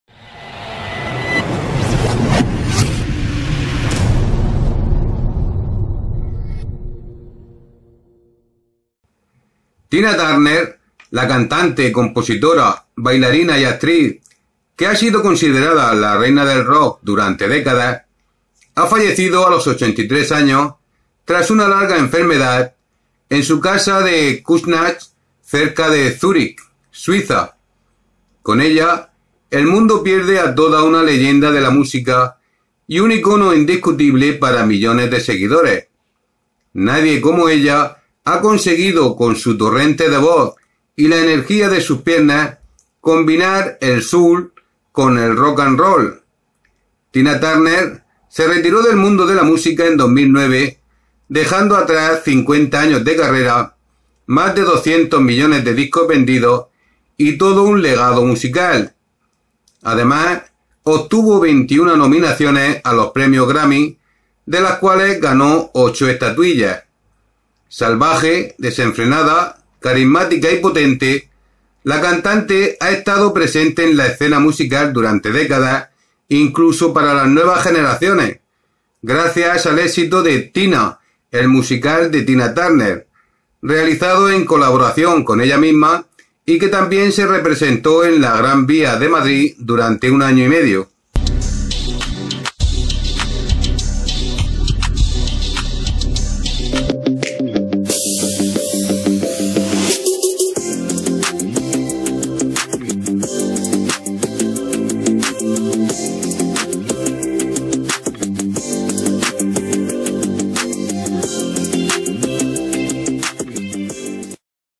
Ha fallecido Tina Turner, la ''Reina del Rock''
Salvaje, desenfrenada, carismática y potente, la cantante ha estado presente en la escena musical durante décadas, siendo considerada como ''Reina del Rock'' y un referente artístico sin igual, incluso por y para las nuevas generaciones.

24/05/2023